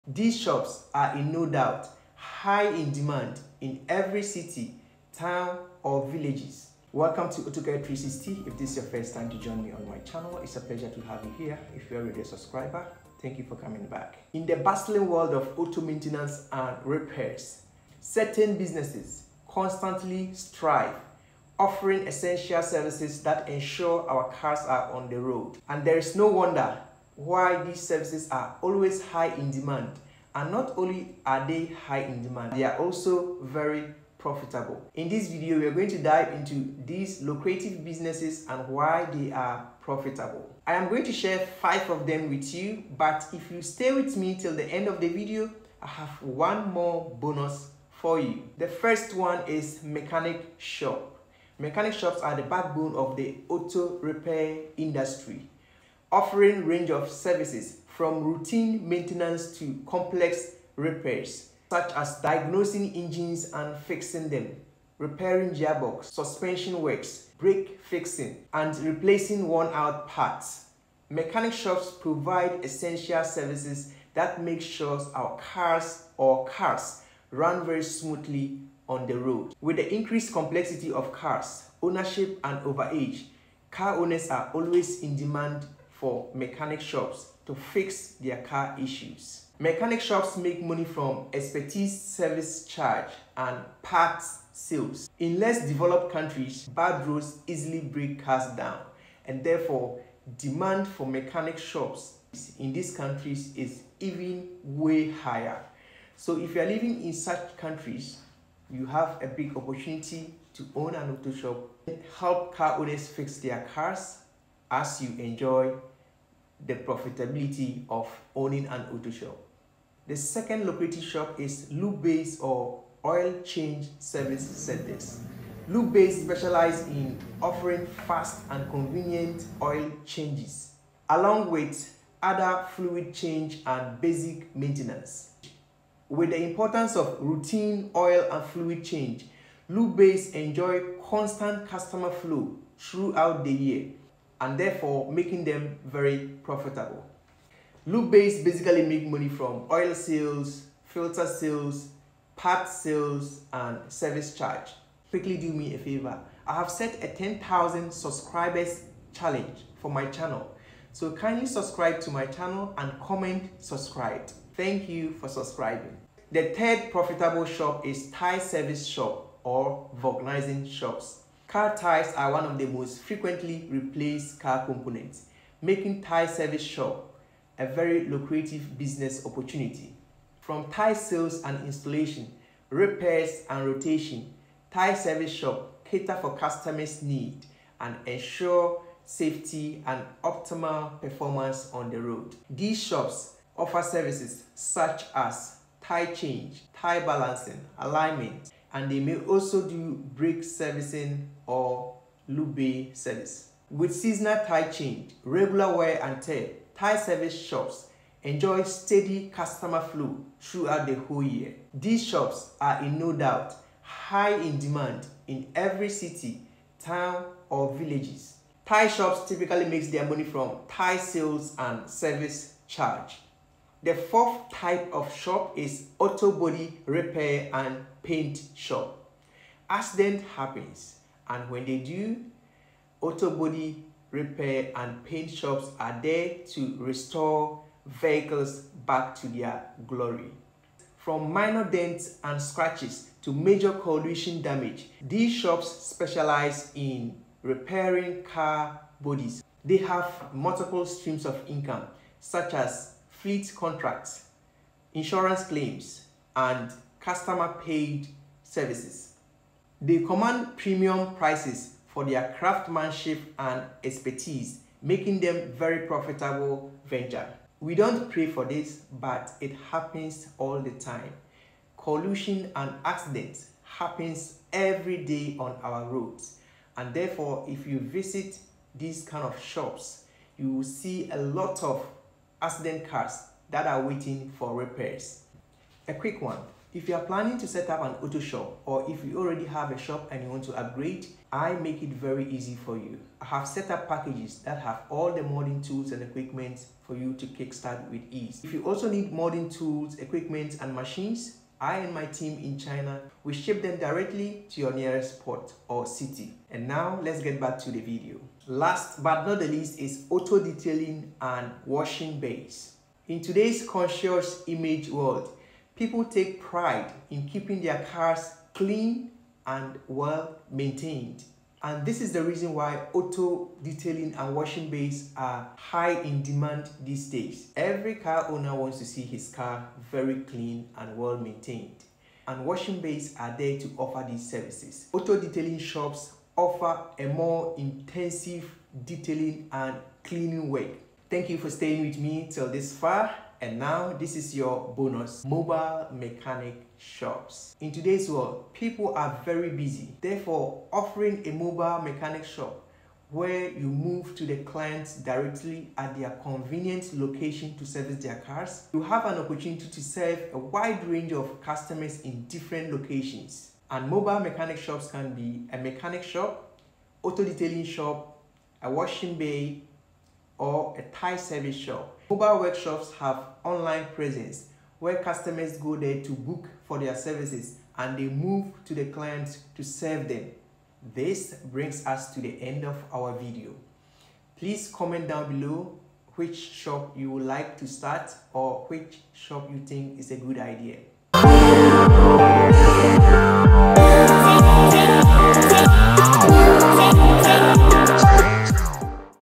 5 Profitable Auto Shops Business That Make Money Daily
In this video, I share five profitable auto shop businesses such as a mechanic shop, Tire service shop, Lube Bay, Car washing bay and detailing etc